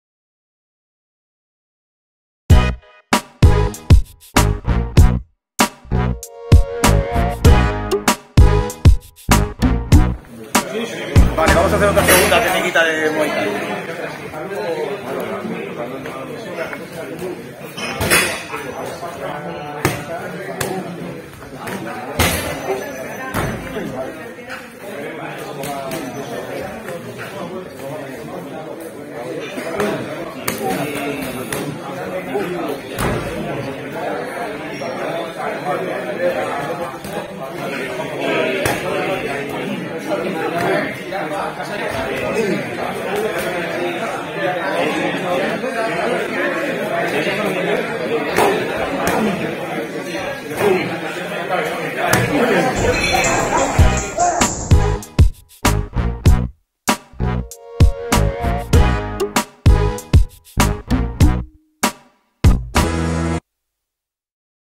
TU ERES EL PROTAGONISTA MUAYTHAI OSCAR GUDIÑA
OSCAR GUDIÑA, DIRECTOR DE MUAYTHAI DE LA FEDERACION MADRILEÑA DE KICKBOXING Y MUAYTHAI, REALIZA UN TRABAJO TECNICO, EN SU CLUB  MADRID BOX.